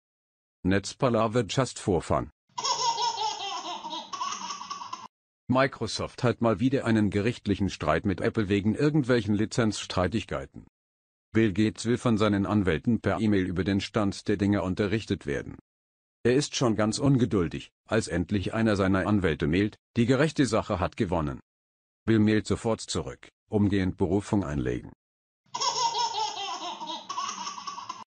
Netzpalaver - Just for fun - Witz 020 - Rechtsstreit mit Apple
"just for fun": Rechtsstreit mit Apple

Microsoft hat mal wieder eine Rechtsstreit mit Apple. Per Mail lässt sich Bill Gates über das Urteil informieren.

Lachen ist Gesund, über sich selber Lachen noch gesünder und schwarzer Humor kann durchaus Medizin sein.